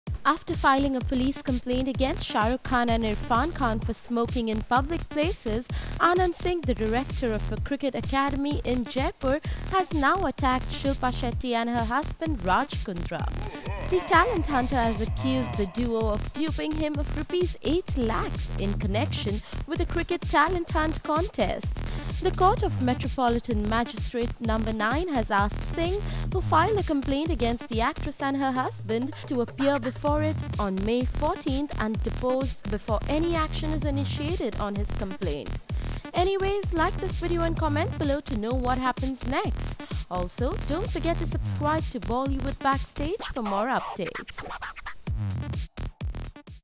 Shilpa Shetty ACCUSED of 8 lakh fraud
Shilpa Shetty ACCUSED of 8 lakh fraud - After filing a police complaint against Shah Rukh Khan and Irrfan Khan for smoking in public places, Anand Singh -- the director of a cricket academy in Jaipur -- has now attacked Shilpa Shetty and her husband Raj Kundra